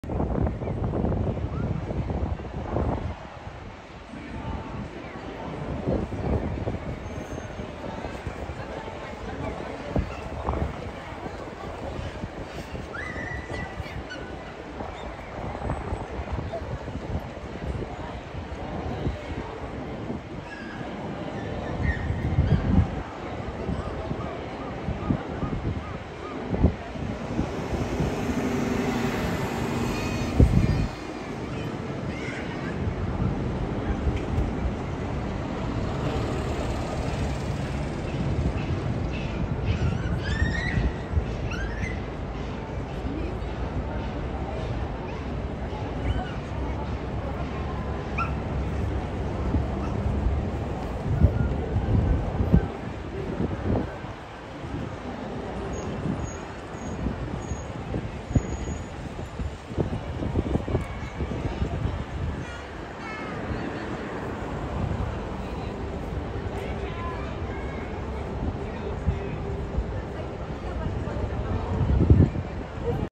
【天気の境目を走るリゾラ】ディズニーリゾートライン Type C ピンク編成 リゾートゲートウェイ・ステーション発車 舞浜駅付近にて
舞浜駅前から、リゾートゲートウェイ・ステーションを
発車するリゾートラインを撮影しました。どうぞお楽しみください！